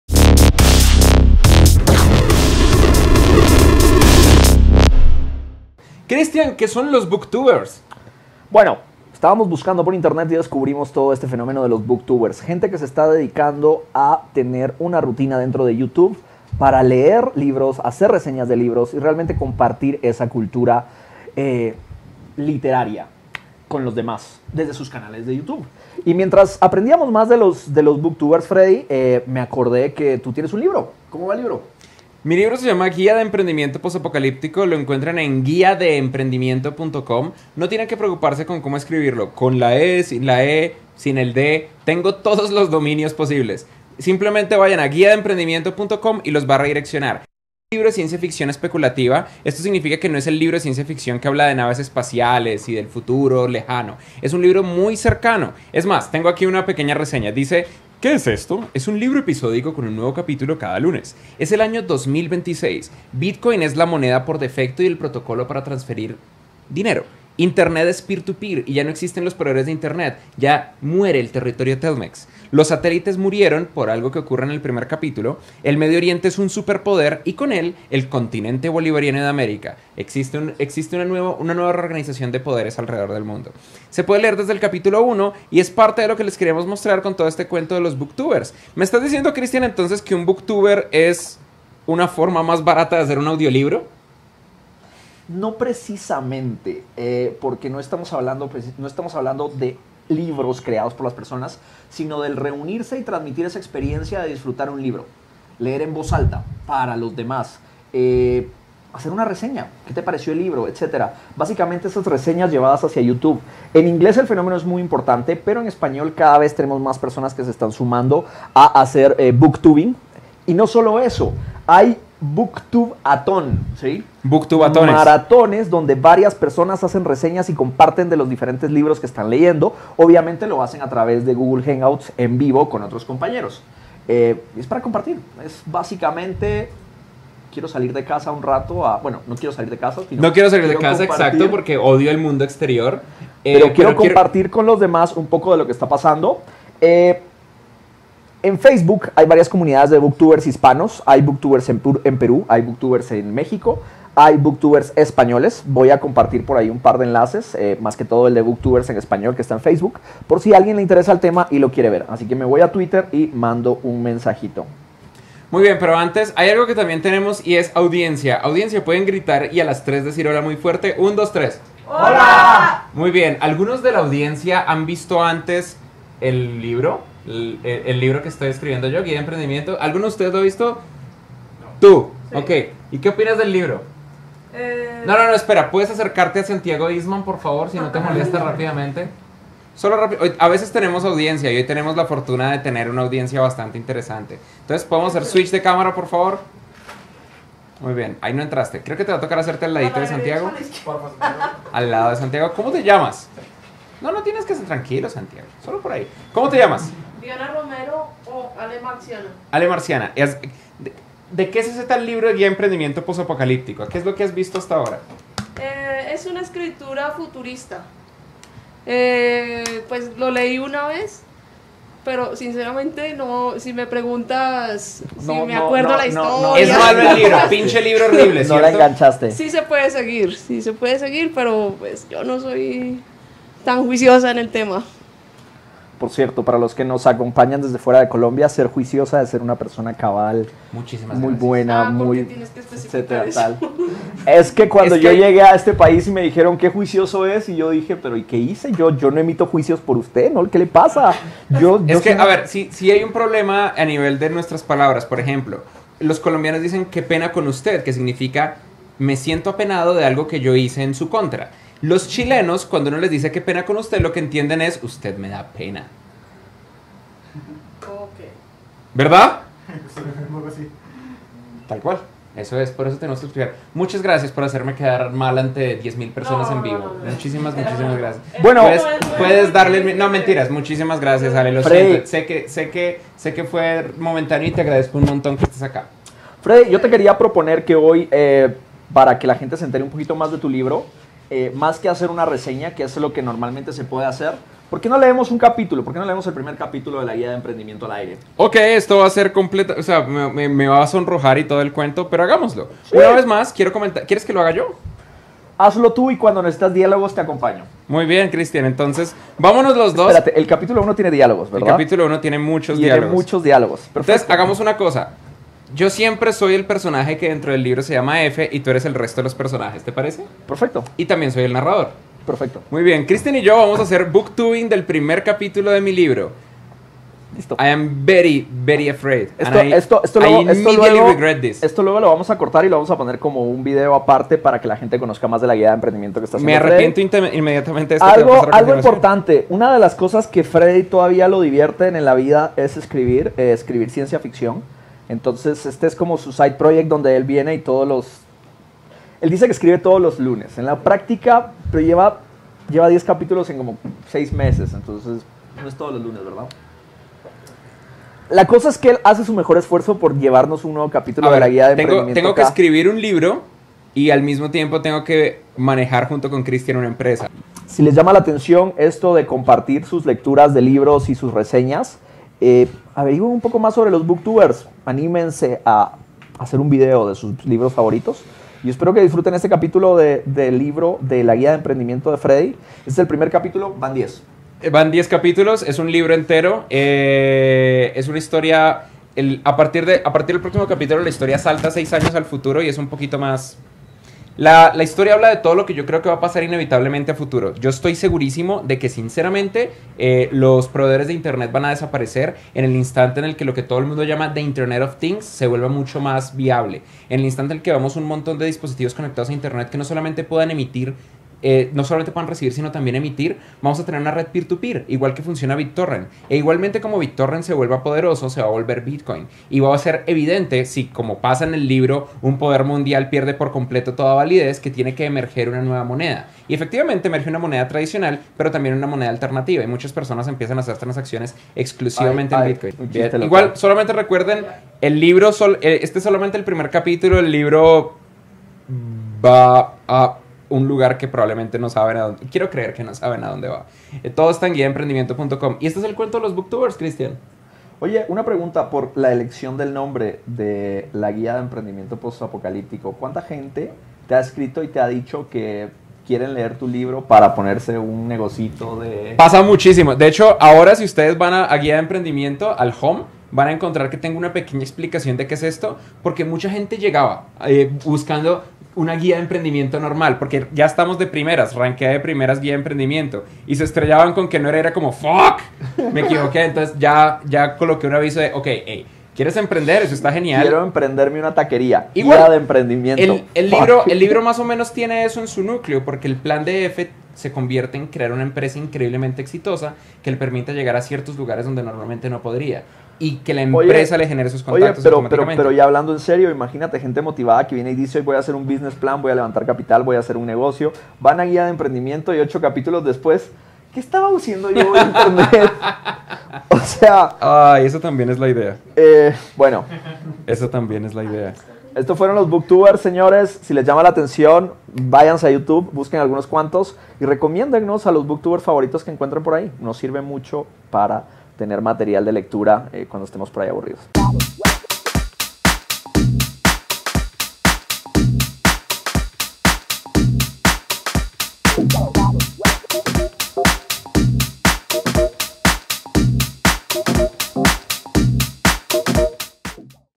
¿Qué es un Booktuber?
En este LIVE, hablamos de los Booktubers, qué y quiénes son, además de la tendencia de conectar libros a Youtube.